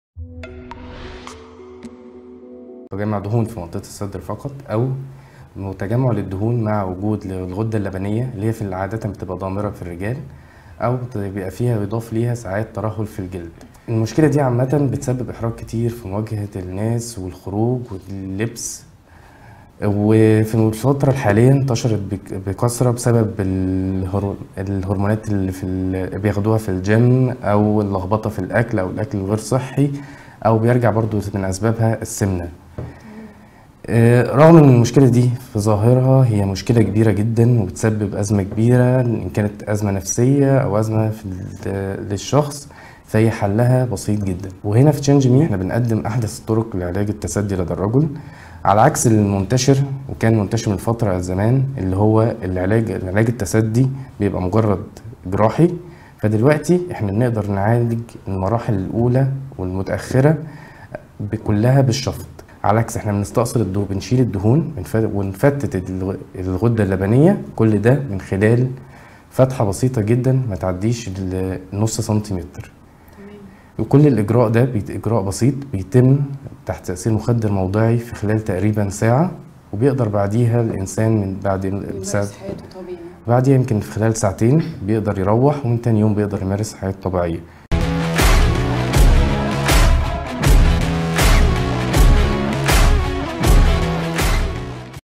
Change me clinic | التثدي لدي الرجال مشكله و حلها في يوم واحد
شاهد كيفية علاج التثدي عند الرجال بدون جراحة بالإضافة إلى تكلفة العملية في عيادة change me في مصر و تعرف اكثر علي اسباب المشكله و امكانيه حلها بأحدث التقنيات حيث يمكنك اختيار افضل دكتور لعلاج التثدي عند الرجال
للحجز و الاستفسار يمكنك الاتصال علي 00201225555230